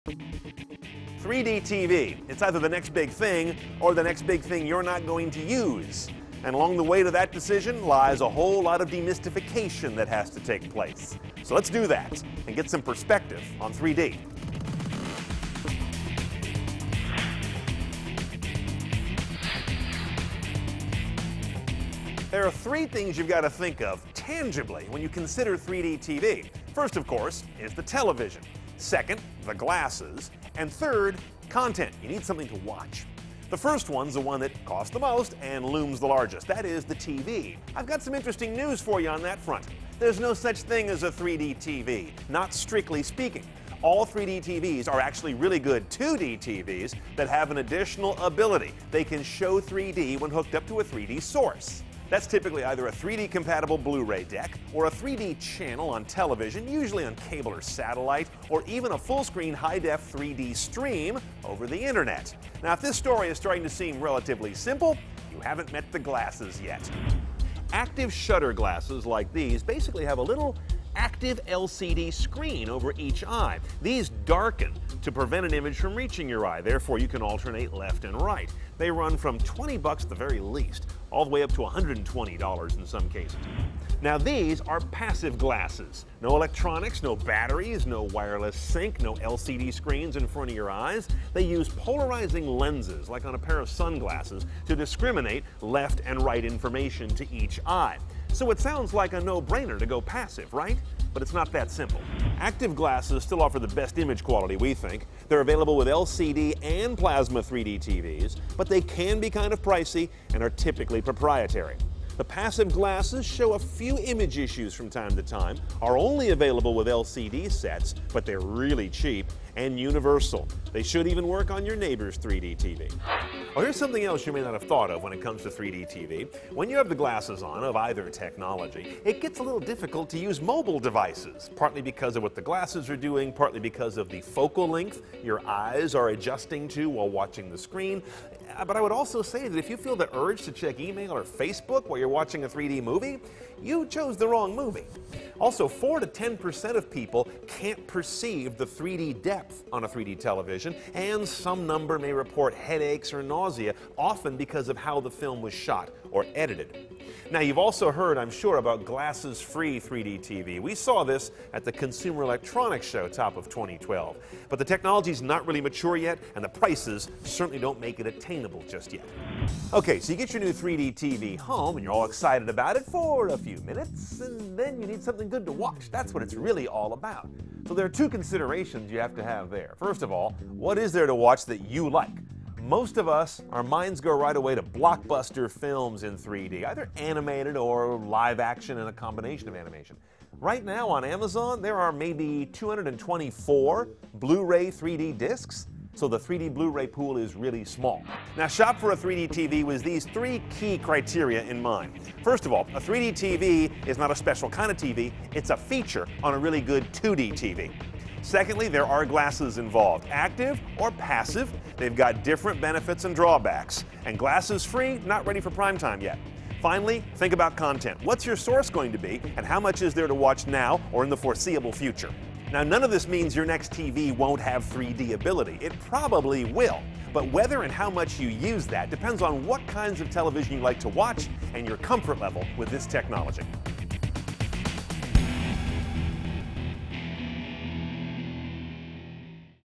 Device & Conquer: Understanding 3D TV
3D TV is everywhere -- but does it make sense in your house?